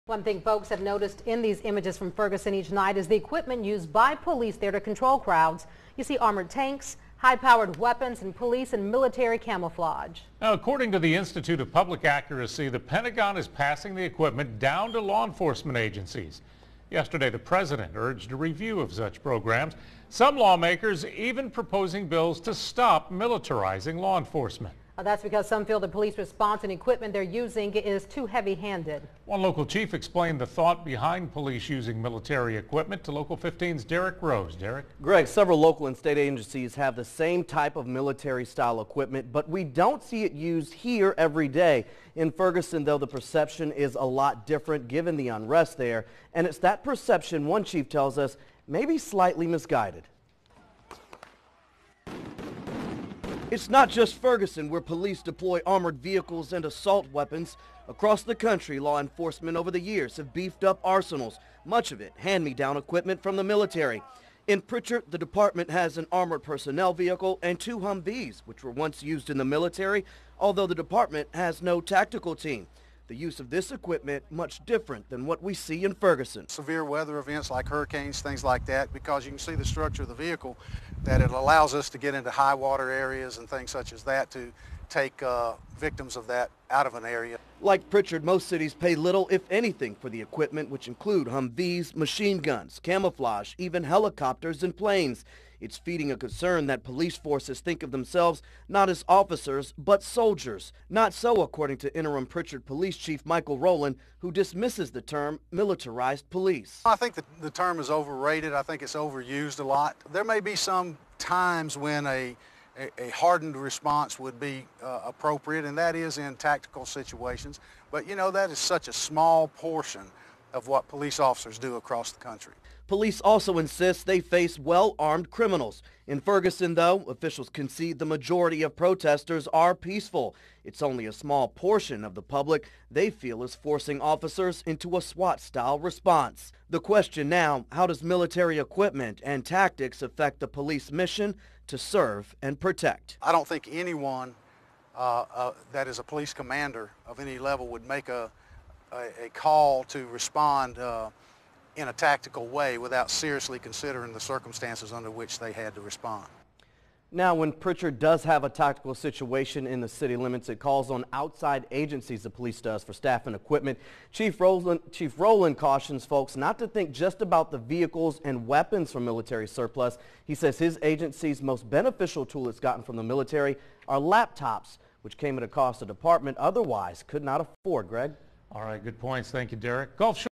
Unrest Highlights Police Use of Military Gear
PRICHARD, Ala. (WPMI/NBC) Because of a trend across the country of police forces beefing up their arsenals with military equipment, the Pentagon insists it has no control over how the equipment is used.

"It is up to law enforcement agencies to speak to how and what they gain through this system," Pentagon Spokesperson Rear Admiral John Kirby said.

Much of the equipment is hand-me-down surplus equipment from the military.

Police in Ft. Myers, Florida are among the many departments that now have a mine-resistant armored vehicle, or MRAP.

"It could sustain handgun, rifle, all the way up to rocket propelled grenades on this vehicle," Ft. Myers Lt. Jeff Bernice told NBC News.

In Prichard, the department has an armored personnel vehicle and 2 Humvees that were used in the military. Both were acquired during the tenure of former Chief Jerry Speziale.

Their use, Interim Chief Michael Rowland said, is much different than what is seen on the streets of Ferguson, Missouri.

"Hurricanes and things like that